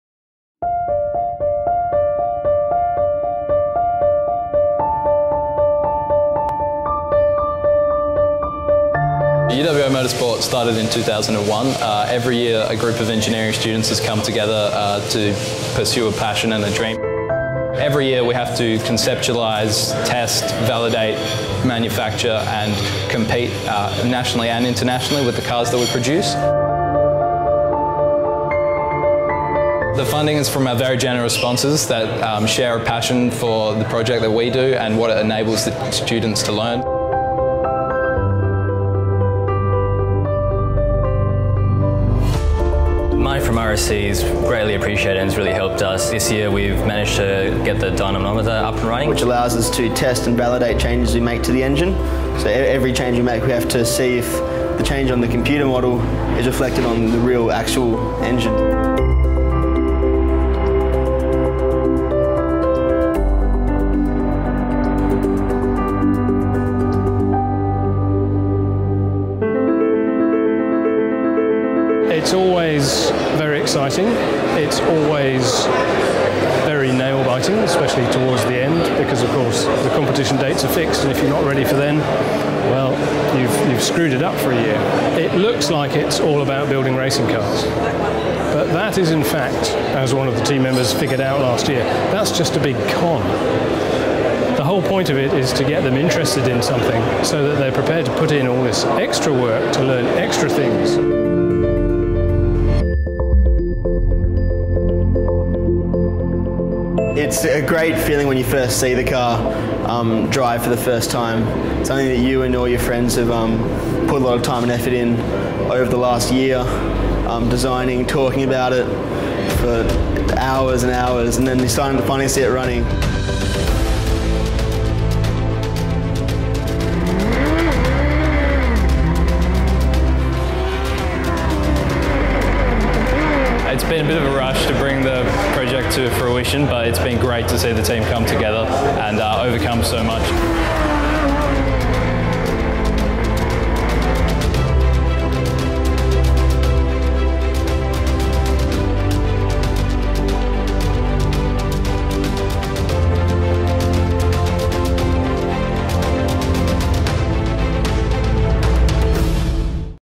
RAC Community Sponsorships @ UWA Motorsport
Having received $15,000 in support through our Community Sponsorship program, the UWA Motorsport team consisting of engineering students developed a greener vehicle focusing on reducing CO₂ emissions and improving fuel economy. The team recently achieved great success at the Australasian Formula SAE competition in Melbourne where they came an astounding fifth place overall and second place in fuel economy out of a total of 23 Universities.